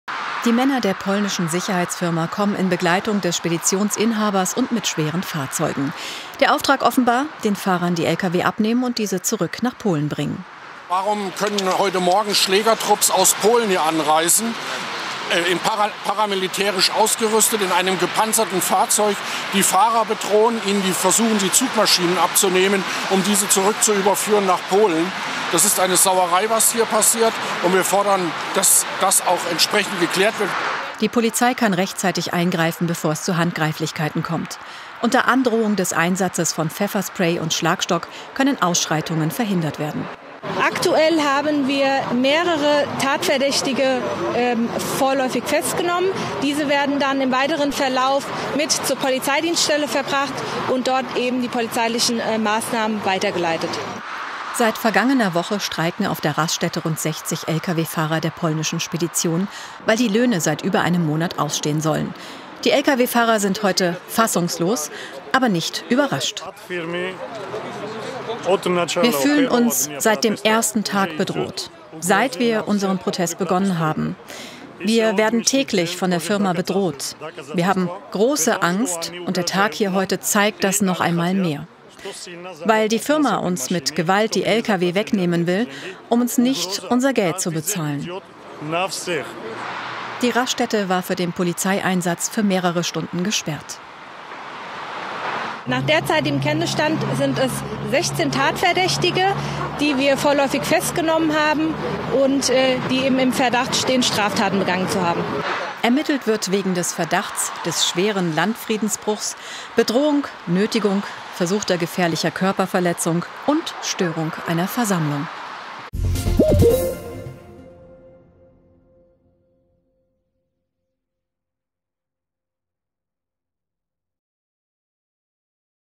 Eskalation auf Raststätte: Streik sollte offenbar mit Gewalt beendet werden | hessenschau
Seit Tagen streiken dutzende Lkw-Fahrer auf einem Rastplatz bei Weiterstadt. Jetzt wollte die zuständige polnische Spedition den Streik offenbar mit Gewalt beenden. Die Polizei musste mit einem Großaufgebot einschreiten, es kam zu Festnahmen.